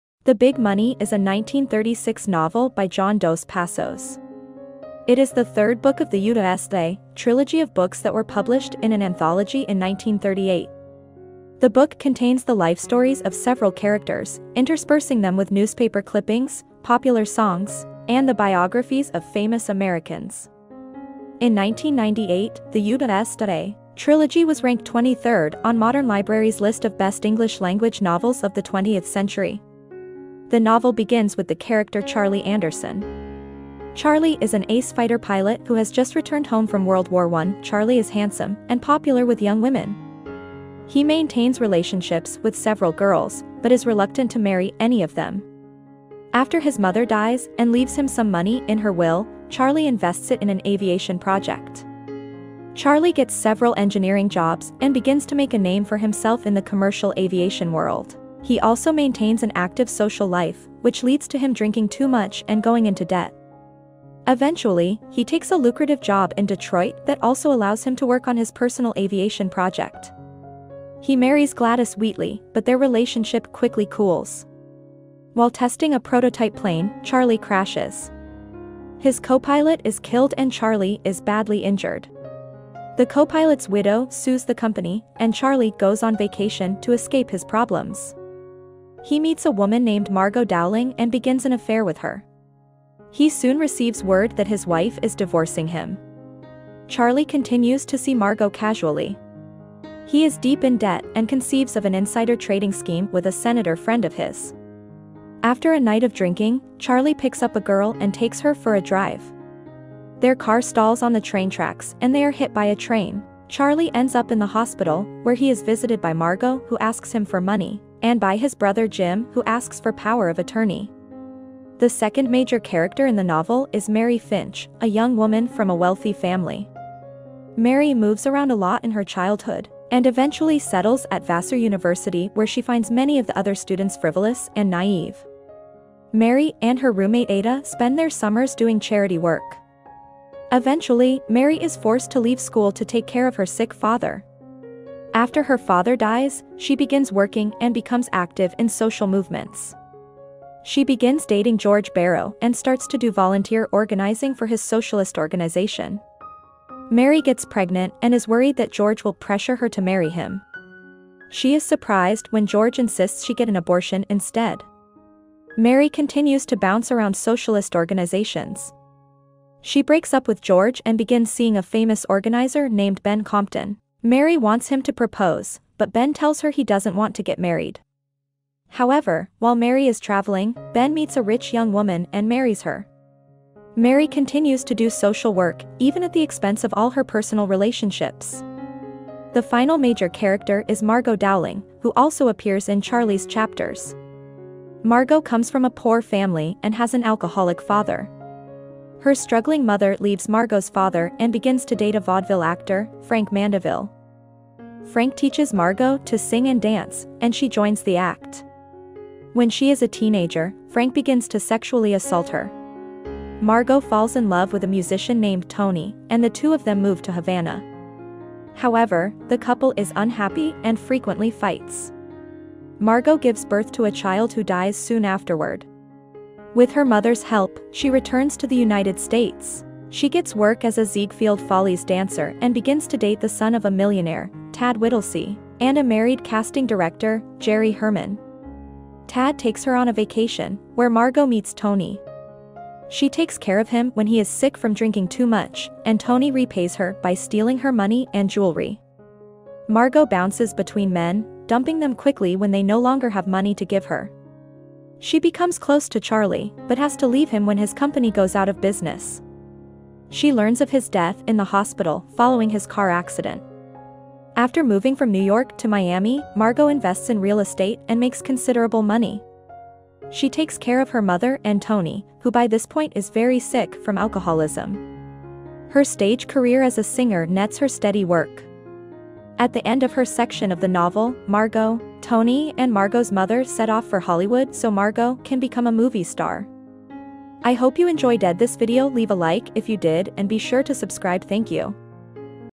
Plot summary, “The Big Money” by John Dos Passos in 5 Minutes - Book Review
The Big Money is a 1936 novel by John Dos Passos. It is the third book of the U.S.A. trilogy of books that were published in an anthology in 1938. The book contains the life stories of several characters, interspersing them with newspaper clippings, popular songs, and the biographies of famous Americans. In 1998, the U.S.A. trilogy was ranked 23rd on Modern Library’s list of Best English Language Novels of the 20th Century.